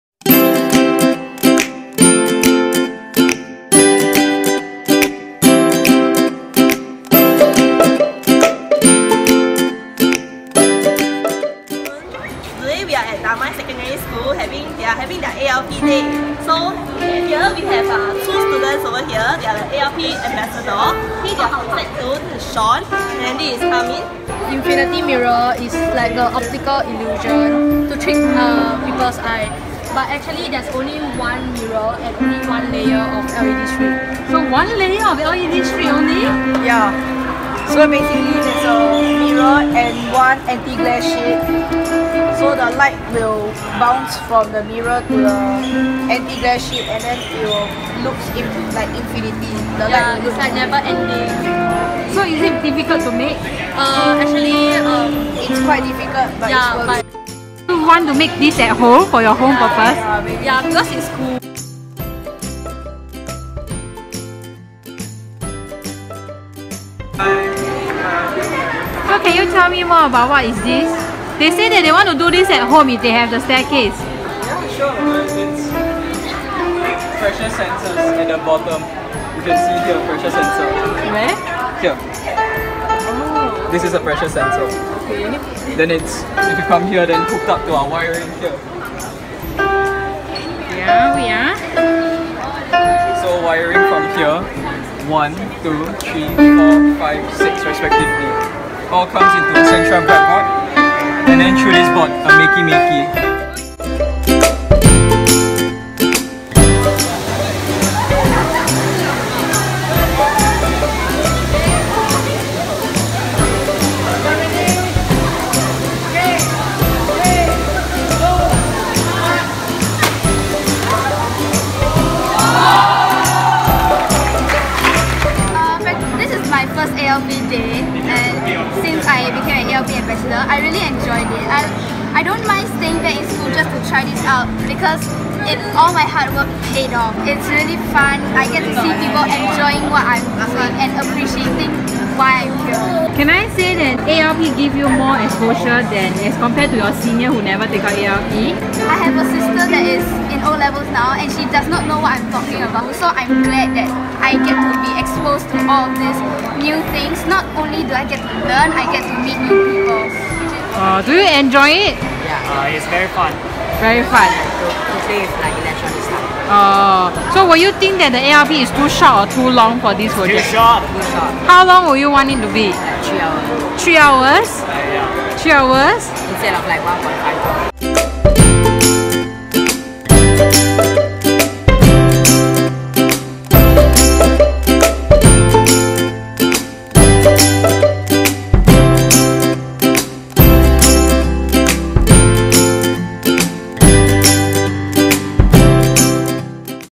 Damai Secondary School Applied Learning Programme (ALP) Day
Damai Secondary School ALP Day held end of 2016. It was run by STEM Ambassadors, students who attended the STEM Applied Learning Programme supported by STEM Inc, Science Centre Singapore.